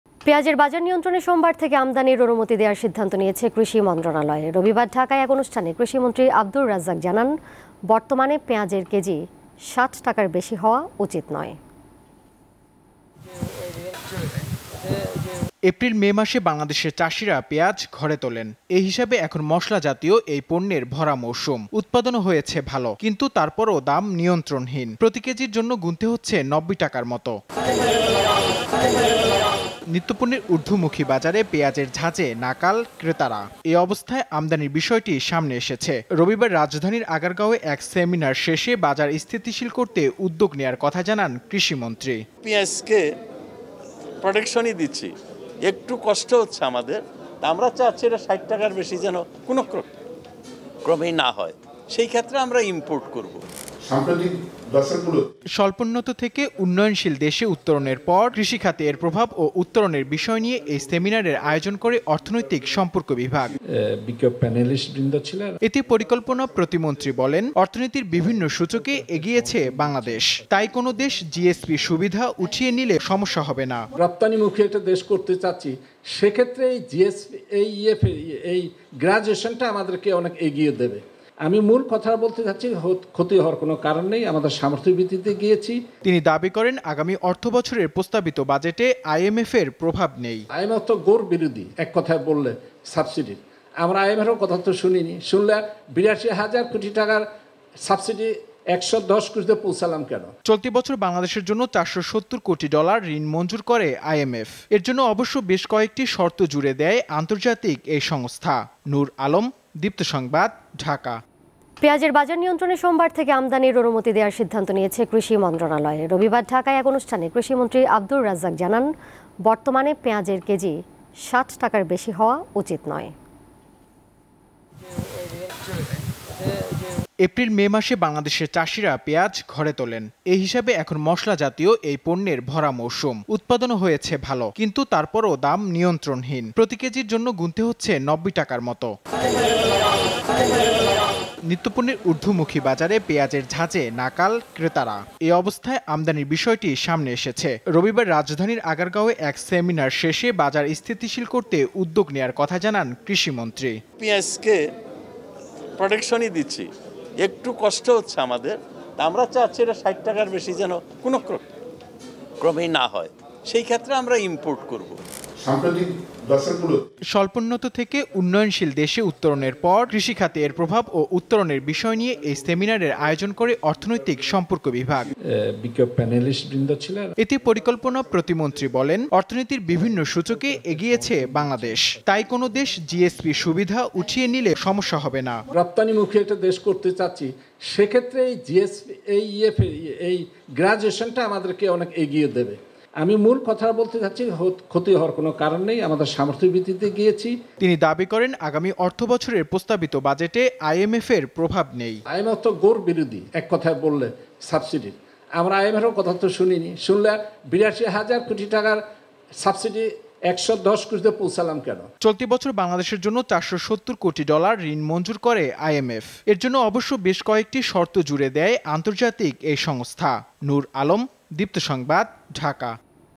সোমবার থেকে পেয়াঁজ আমদানির অনুমতি, থাকবে না সংকট || Onion import || Deepto News
সোমবার থেকে পেয়াঁজ আমদানির অনুমতি, থাকবে না সংকট

Address:
Deepto TV
7/A/GA Tejgaon Industrial Area,